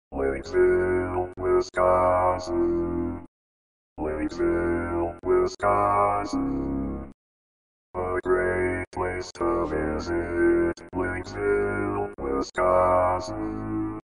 How to Say or Pronounce USA Cities — Lynxville, Wisconsin
This video shows you how to say or pronounce Lynxville, Wisconsin.

A computer said Lynxville, Wisconsin. How would you say Lynxville, Wisconsin?